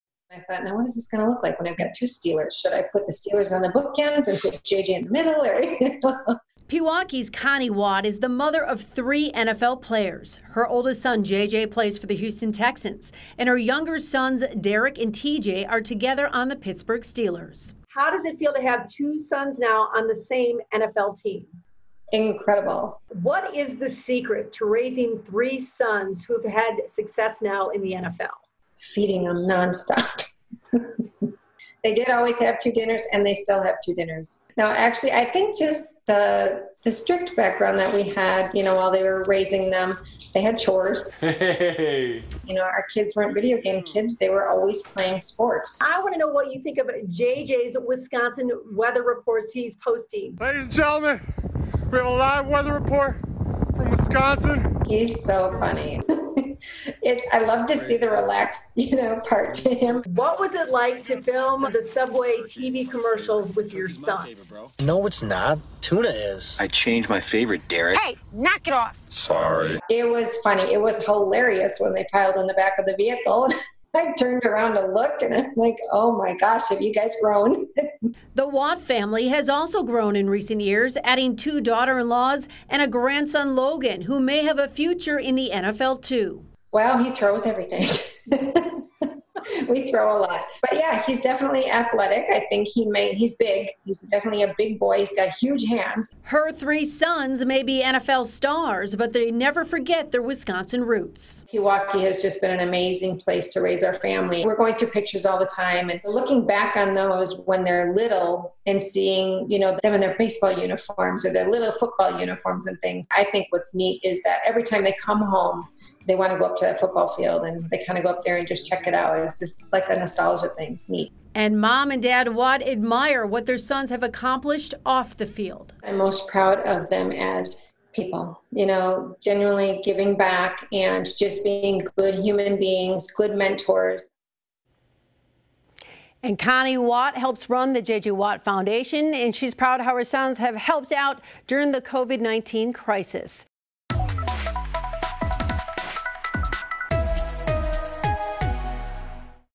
Watt a mom! Connie Watt shares secret of raising three sons in NFL
Connie Watt is the ultimate sports mom with three sons playing in the NFL.